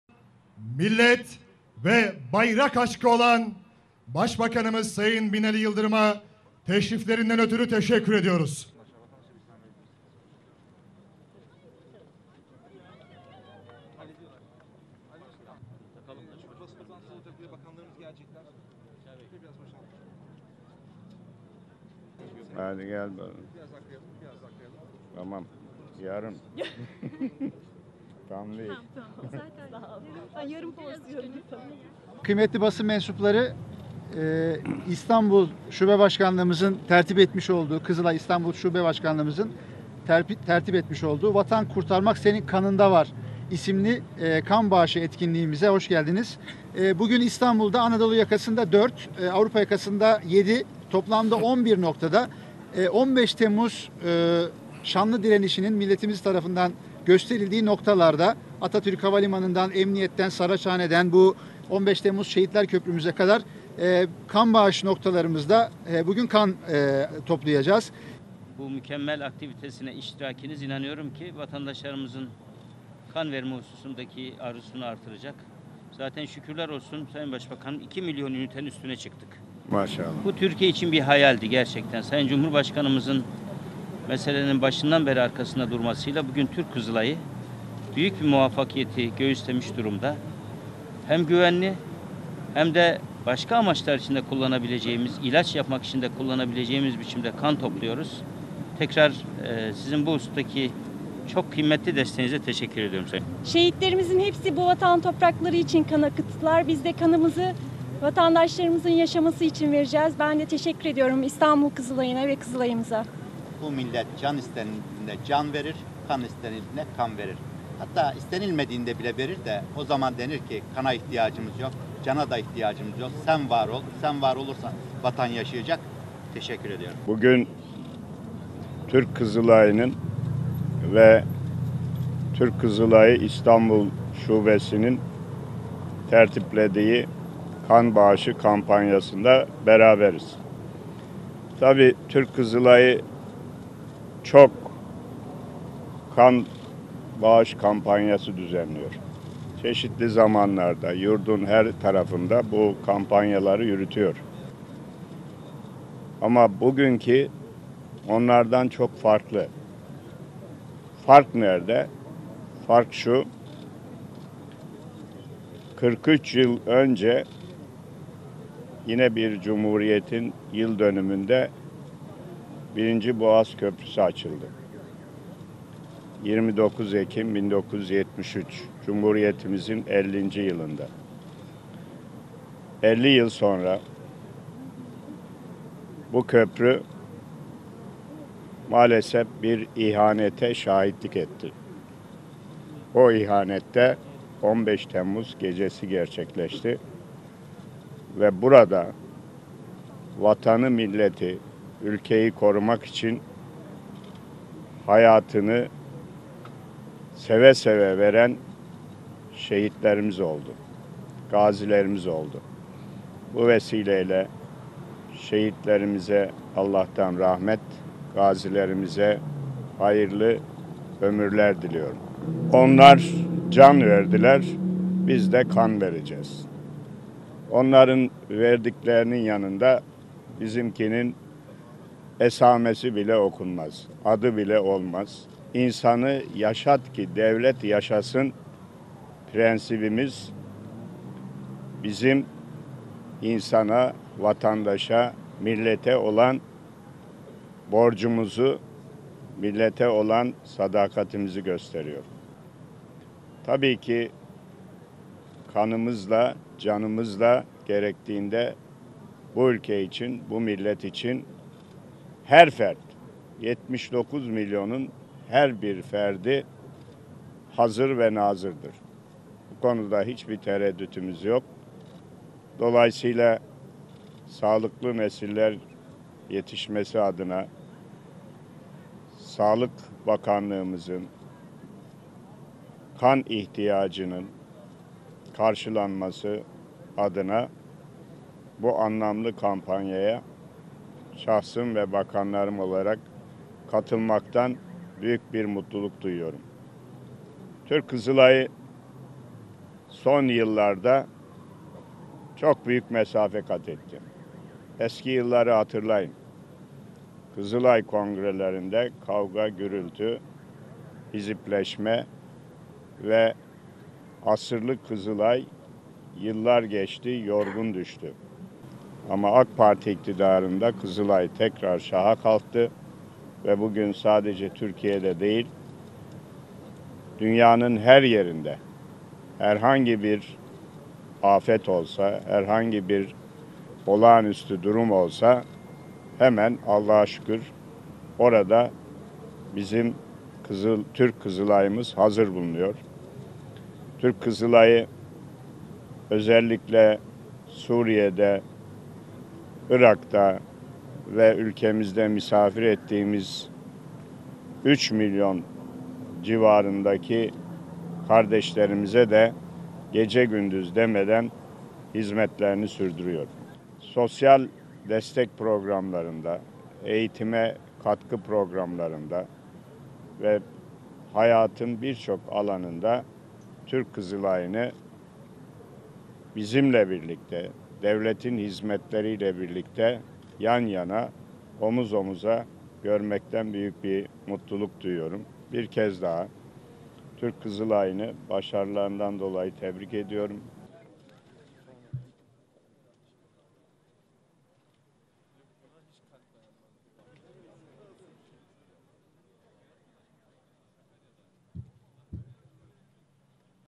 Başbakan Yıldırım: 'Onlar Can Verdi, Biz De Kan'
Başbakan Binali Yıldırım Türk Kızılayı’nın ’Vatan Sana Kanım Feda’ başlıklı kampanyası kapsamında 15 Temmuz Şehitler Köprüsü’nde kan verdi. 15 Temmuz şehitlerini hatırlatan Başbakan Yıldırım, "Onlar can verdiler, biz de kan vereceğiz. Onların verdiklerinin yanında bizimkinin esamesi bile okunmaz" dedi.

Türk Kızılayı İstanbul Şubesi, 15 Temmuz gecesi darbecilerin kan döktüğü 11 noktada kan bağışı kampanyası başlattı. ’Vatan Sana Kanım Feda’ sloganıyla düzenlenen kan bağışı kampanyası kapsamında 15 Temmuz Şehitler Köprüsü’nde de kan bağışı çadırı kuruldu.

Başbakan Binali Yıldırım da Türk Kızılayı çadırını ziyaret ederek kampanyaya destek verdi. Başbakan Yıldırım 15 Temmuz Şehitler Köprüsü’nde Sağlık Bakanı Recep Akdağ, Milli Eğitim Bakanı İsmet Yılmaz, Aile ve Sosyal Politikalar Bakanı Fatma Betül Sayan Kaya, İstanbul Valisi Vasip Şahin, AK Parti İstanbul İl Başkanı Selim Temurci, Fatih Belediye Başkanı Mustafa Demir, Sultanbeyli Belediye Başkanı Hüseyin Keskin, Üsküdar Belediye Başkanı Hilmi Türkmen, Çekmeköy Belediye Başkanı Ahmet Poyraz, Türk Kızılayı Genel Başkanı Dr. Kerem Kınık ve vatandaşlar tarafından karşılandı.

Kan bağışı etkinliğinde konuşan Türk Kızılayı Genel Başkanı Kerem Kınık, "İstanbul Şube Başkanlığımızın tertip etmiş olduğu ‘Vatan Kurtarmak Senin Kanında Var’ isimli kan bağışı etkinliğimize hoşgeldiniz. Bugün İstanbul Anadolu yakasında 4, Avrupa noktasında 7 toplam 11 noktada, 15 Temmuz direnişinin gösterildiği noktalarda kan toplayacağız" ifadelerini kullandı.

Sağlık Bakanı Recep Akdağ ise Türk Kızılayı’nın 2 milyon ünite kan bağışı rakamına ulaştığını hatırlatarak, "Bu Türkiye için bir hayaldi. Bugün Türk Kızılayı hem ihtiyaç için, hem de başka amaçlar için, ilaç yapmak için kullanabilecek şekilde kan topluyoruz. Desteğinize teşekkür ediyorum" diye konuştu.

Törende konuşan Aile ve Sosyal Politikalar Bakanı Fatma Betül Sayan Kaya da "Bu köprüde 39 şehidimiz canını, kanını vatan toprakları için akıttı. Biz de insanlarımızın yaşaması için kan bağışında bulunacağız" ifadelerini kaydetti.

Milli Eğitim Bakanı İsmet Yılmaz da "Bu milletin can istendiğinde can verir, kan istendiğinde kan verir" dedi.

"ONLAR CAN VERDİLER, BİZ DE KAN VERECEĞİZ"

Başbakan Binali Yıldırım da konuşmasına Türk Kızılayı’nı tebrik ederek başladı.

Türk Kızılayı’nın çeşitli zamanlarda kan bağışı kampanyası düzenlediğini ancak bu seferki kampanyanın çok farklı olduğunu vurgulayan Yıldırım, "Fark şu. 43 yıl önce yine bir Cumhuriyet’in yıldönümünde 1. Boğaz Köprüsü açıldı. 29 Ekim 1973. Cumhuriyetimizin 50’nci yılında. 50 yıl sonra bu köprü maalesef bir ihanete şahitlik etti. O ihanet de 15 Temmuz gecesi gerçekleşti. Burada vatanı milleti korumak için hayatını seve seve veren şehitlerimiz oldu, gazilerimiz oldu. Bu vesile ile şehitlerimize Allah’tan rahmet, gazilerimize hayırlı ömürler diliyorum. Onlar can verdiler, biz de kan vereceğiz. Onların verdiklerinin yanında bizimkinin esamesi bile okunmaz. Ama ‘insanı yaşat ki devlet yaşasın’ prensibimiz bizim vatandaşa olan borcumuzu, sadakatimizi gösteriyor. Tabii ki kanımızla, canımızla gerektiğinde bu ülke için her fert hazır ve nazırdır. Bu konuda tereddüdümüz yok. Dolayısıyla sağlıklı nesiller yetişmesi adına Sağlık Bakanlığımızın kan ihtiyacının karşılanması adına bu anlamlı kampanyaya şahsım ve bakanlarım olarak katılmaktan büyük mutluluk duyuyorum" şeklinde konuştu.

Türk Kızılayı’nın son yıllarda büyük mesafeler kat ettiğini belirten Yıldırım, şunları söyledi:

"Eski yılları hatırlayın. Kızılay kongrelerinde kavga gürültü, asırlık Kızılay yıllar geçti yorgun düştü. AK Parti iktidarında Kızılay tekrar şaha kalktı ve dünyanın herhangi bir yerinde afet olsa hemen Allah’a şükür orada bizim Türk Kızılayı’mız hazır bulunuyor. 3 milyon misafir kardeşlerimize de gece gündün hizmetlerini sürdürüyor. Sosyal destek programlarında, eğitime katkı programlarında, hayatın birçok alanında Türk Kızılayı’nı bizimle birlikte yan yana görmekten büyük mutluluk duyuyorum. Bir kez daha Türk Kızılayı’nı tebrik ediyorum"

Konuşmaların ardından Başbakan Binali Yıldırım İstanbul genelindeki kan bağışı noktalarına telefonla canlı bağlanarak çalışmalar hakkında bilgi aldı. Başbakan Yıldırım daha sonra ise kan vermek üzere Kızılay çadırına gitti.